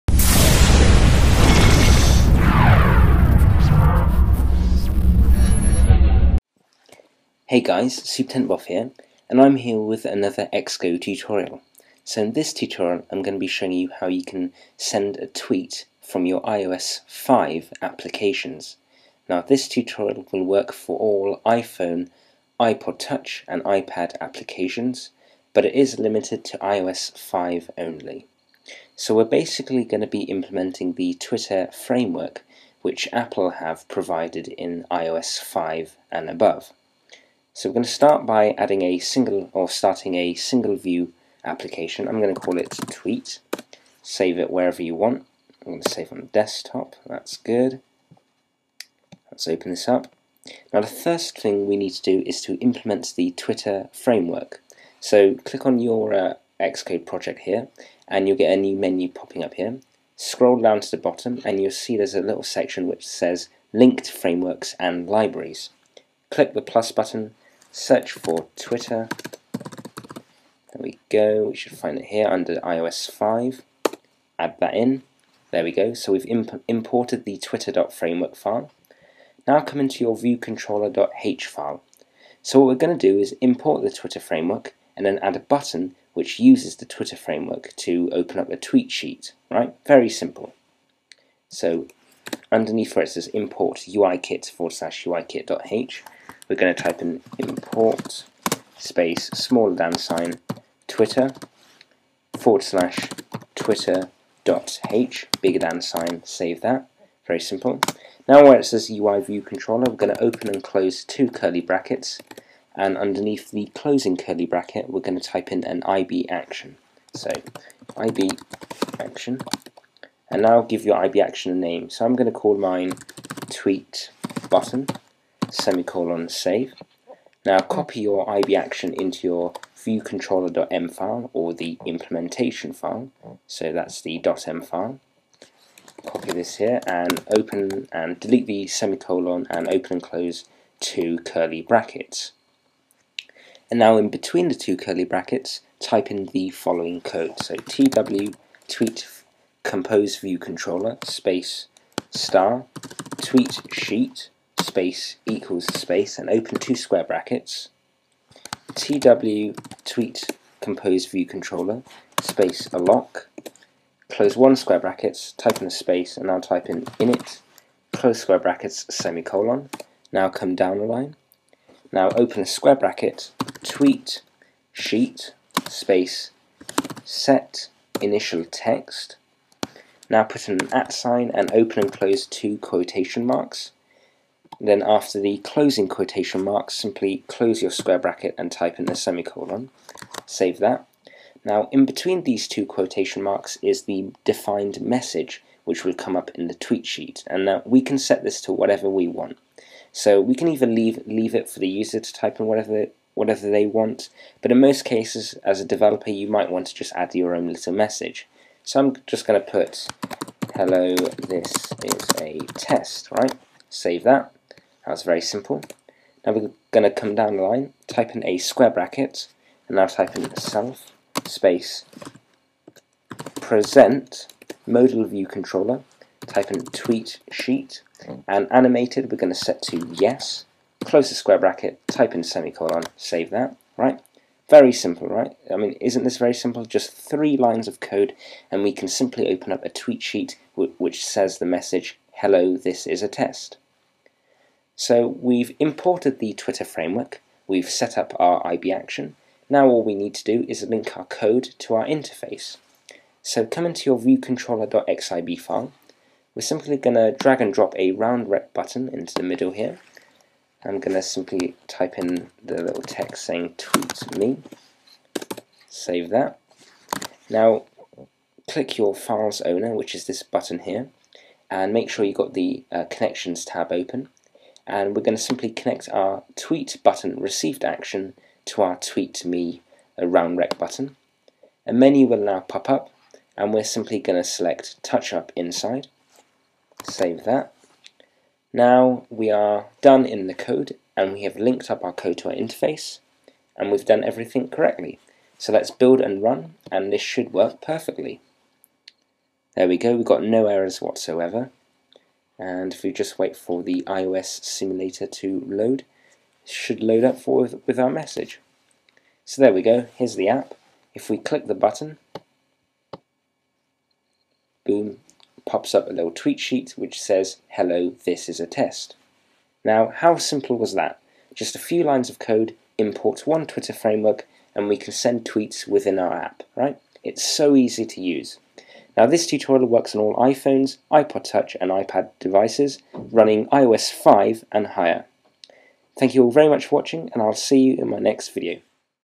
Xcode Tutorial - Send a Tweet - iOS 5
In this Xcode tutorial I will be showing you how you can send a Tweet from within a iOS 5 application.